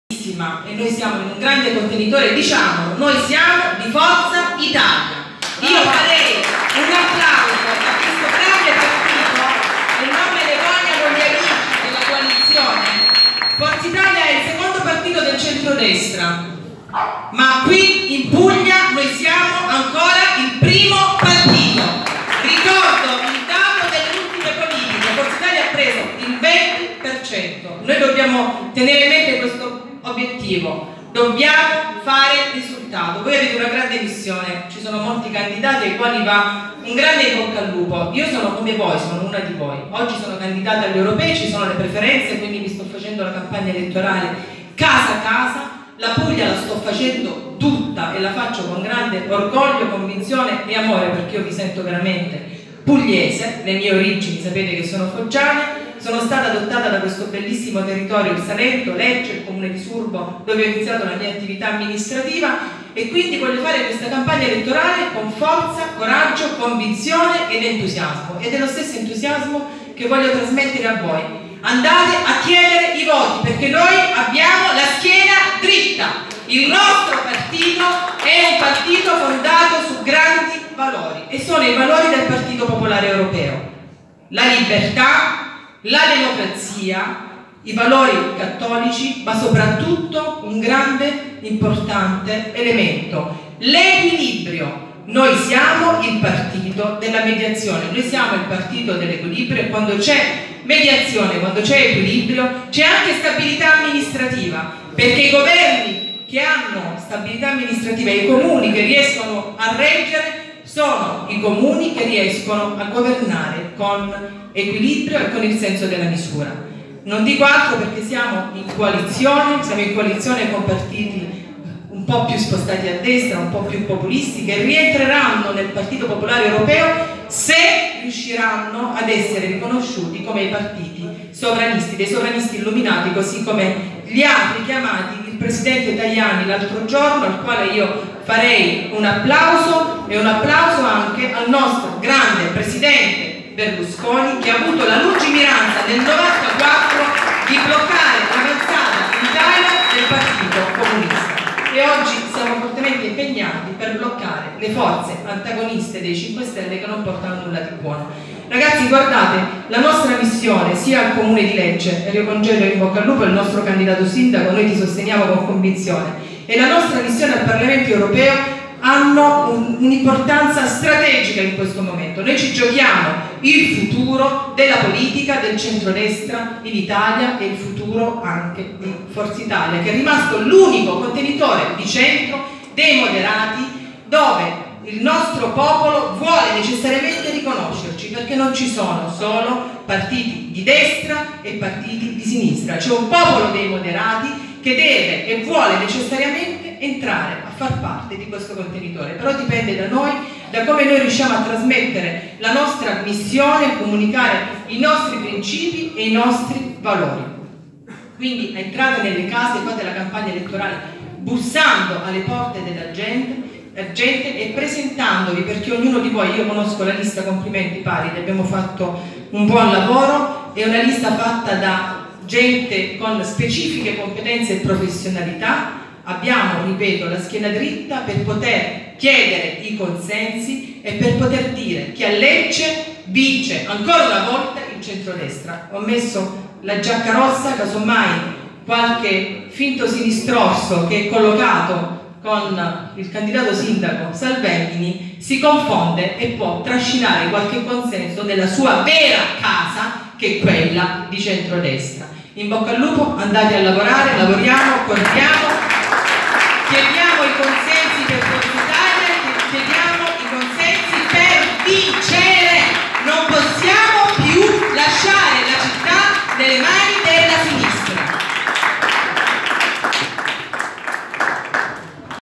ELEZIONI EUROPEE 2019 - INTERVENTO FILOMENA D'ANTINI (Forza Italia) - Leccenews24
Notizie chiare e puntuali, informazioni a misura di ciascun lettore, approfondimenti continui 24 ore su 24 da Lecce e dal Salento.